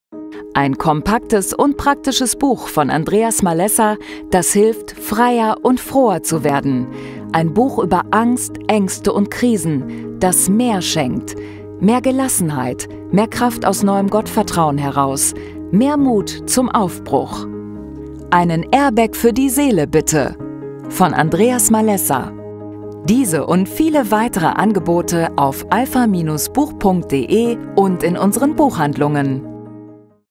Einen Airbag für die Seele - Bucheinblick
"Einen Airbag für die Seele, bitte!", ist ein kompaktes, konkretes und praktisches Buch zum Thema Angst, Ängste, Krisen - das den Leser freier, froher und mutiger macht. Unkompliziert, lebensnah und entlastend ehrlich geschrieben. Eine tiefe Vertrauenserklärung an Gott, fast wie ein Glaubensbekenntnis - bei allen Unwägbarkeiten des Lebens. Von einem Autor verfasst, der geistliche Tiefe und Weite mit einem exzellenten Schreibstil verbindet. Ein Buch wie Andreas Malessa selbst: kompetent, offen, ehrlich - und immer mit Stil und entlastendem Humor. Ein Buch, das MEHR schenkt: Mehr Mut zum Aufbruch. MEHR Kraft - aus neuem Gottvertrauen heraus. MEHR Gelassenheit - weil "Gott weiß" und weil er der "Gott mit uns" ist: Immanuel.